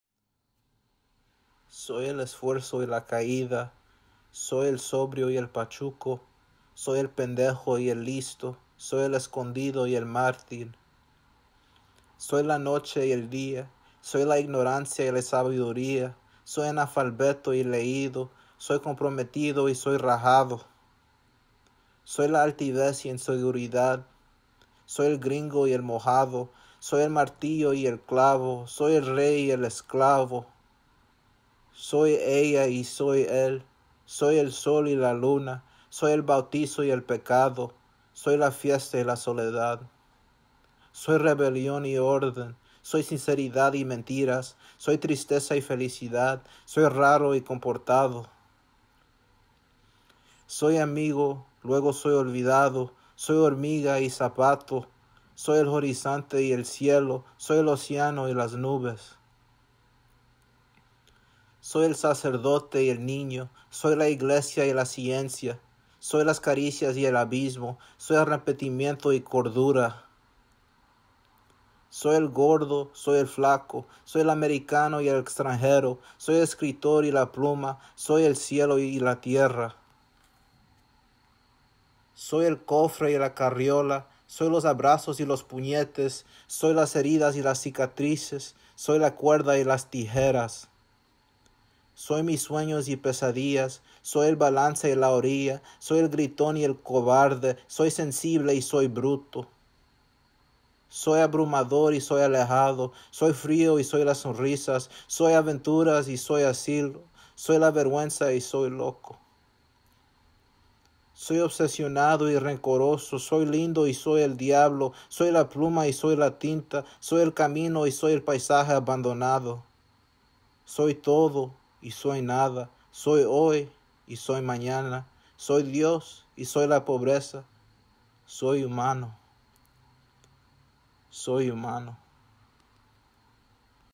SOY HUMANO - POETRY & FILM PHOTOGRAPHY
Written By Luis Cuevas

Double Exposures Shot By Luis Cuevas

Soy Humano -

Soy el esfuerzo y la caída
Soy el sobrio y el pachuco
Soy el pendejo y el listo
Soy el escondido y el mártir

Soy la noche y el día
Soy la ignorancia y la sabiduría
Soy analfabeto y leído
Soy comprometido y soy rajado

Soy la altivez e inseguridad
Soy el gringo y el Mojado
Soy el martillo y el clavo
Soy el rey y el esclavo

Soy ella y soy el
Soy el sol y la Luna
Soy el bautizo y el pecado
Soy la fiesta y la soledad

Soy rebelión y orden
Soy sinceridad y mentiras
Soy tristeza y felicidad
Soy raro y comportado

Soy amigo luego soy olvidado
Soy hormiga y zapato
Soy el horizonte y el cielo
Soy el océano y las nubes

Soy el sacerdote y el niño
Soy la iglesia y las ciencias
Soy las caricias y el abismo
Soy arrepentimiento y la cordura

Soy el gordo soy el flaco
Soy el americano y el extranjero
Soy escritor y la pluma
Soy el cielo y la tierra

Soy el cofre y la carriola
Soy los abrazos y los puñetes
Soy las heridas y las cicatrices
Soy la cuerda y soy las tijeras

Soy mis sueños y pesadillas
Soy el balance y la orilla
Soy el gritón y el cobarde
Soy sensible y soy bruto

Soy abrumador y soy alejado
Soy frio y soy las sonrisas
Soy aventuras y soy asilo
Soy la vergüenza y soy loco

Soy obsesionado y rencoroso
Soy lindo y soy el diablo
Soy la pluma y soy la tinta
Soy el camino y soy el paisaje abandonado

Soy todo y soy nada
Soy hoy y soy Mañana
Soy dios y soy la pobreza
Soy humano, Soy humano